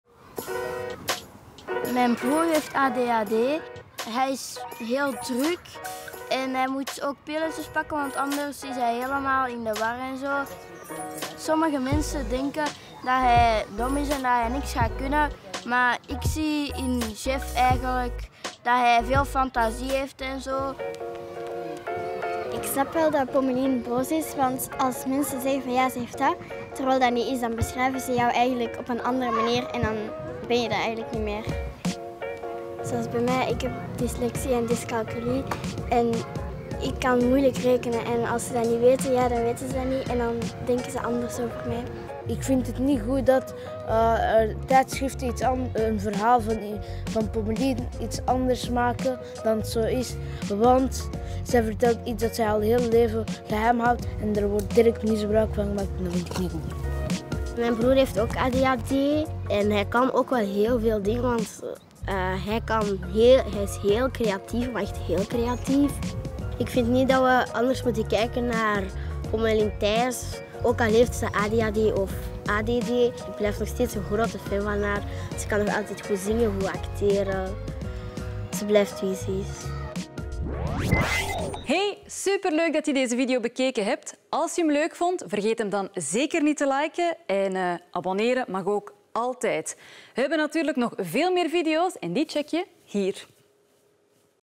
Pommelien Thijs praat voor het eerst over haar ADD
Pommelien Thijs heeft in een interview gezegd dat ze de aandachtsstoornis ADD heeft, maar een tijdschrift heeft daar iets anders van gemaakt, waar ze niet mee akkoord is. En nu is ze boos. Nu, ADD is een aandachtstoornis en een vorm van ADHD... Maar het grote verschil tussen de twee is dat mensen met ADHD vaak druk of onrustig zijn en bij ADD is dat niet altijd zo. Maar hoe is het nu om dat zelf te hebben?